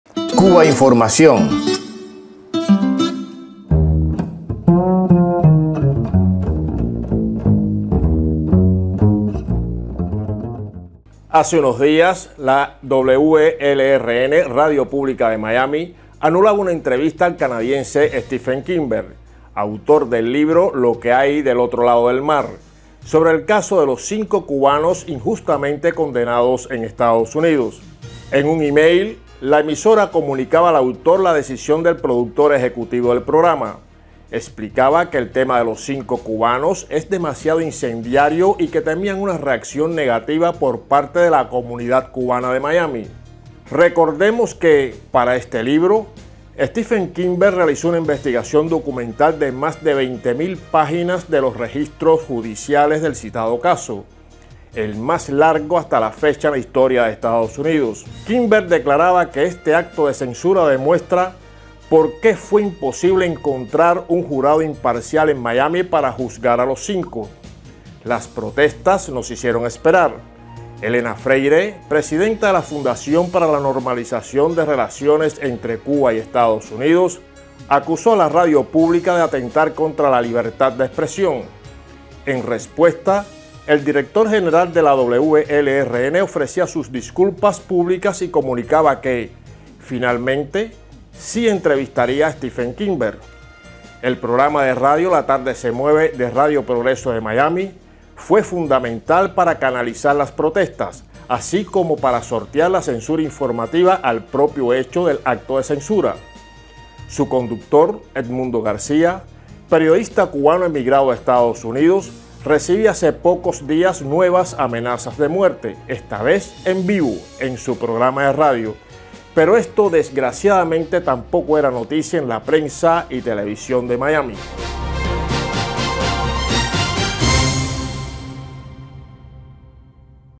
Protestas hacen que radio pública de Miami sí entreviste finalmente a autor de libro sobre Los Cinco
Cubainformación TV -- Basado en un texto de Edmundo García -- "La tarde se mueve" (Miami) / Blog "La pupila insomne" (Cuba).- Hace unos días, la WLRN (radio pública de Miami) anulaba una entrevista al canadiense Stephen Kimber, autor del libro "Lo que hay del otro lado del Mar", sobre el caso de los cinco cubanos injustamente condenados en EEUU. En un email, la emisora comunicaba al autor la decisión del productor ejecutivo del programa. Explicaba que el tema de Los Cinco cubanos es "demasiado incendiario" y que temían una reacción negativa por parte de la "comunidad cubana de Miami".